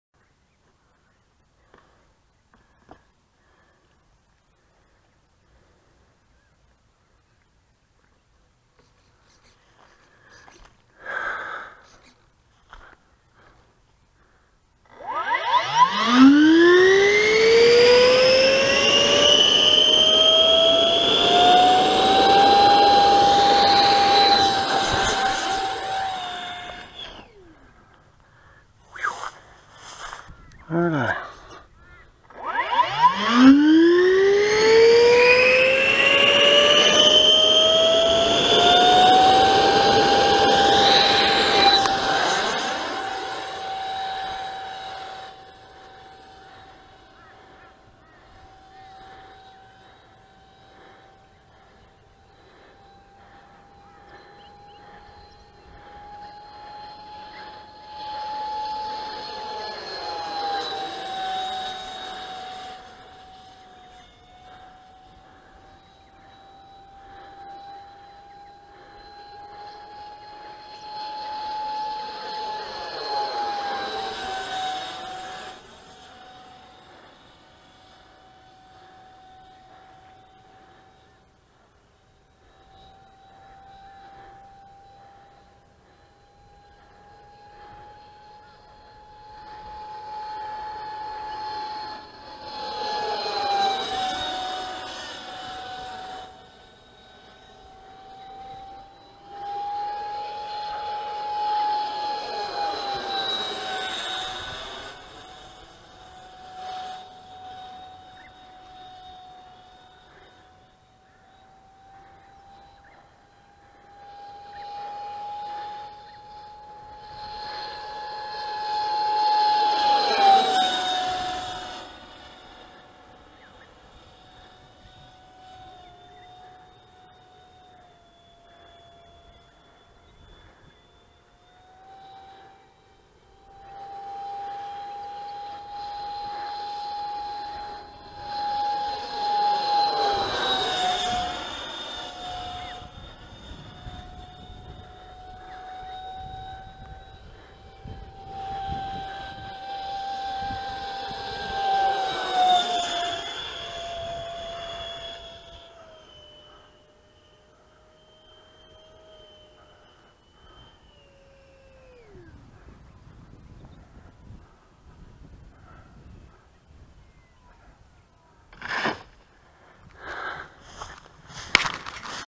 Rc plane BAE Hawk Maiden
Came through with no damage. needs some small mods.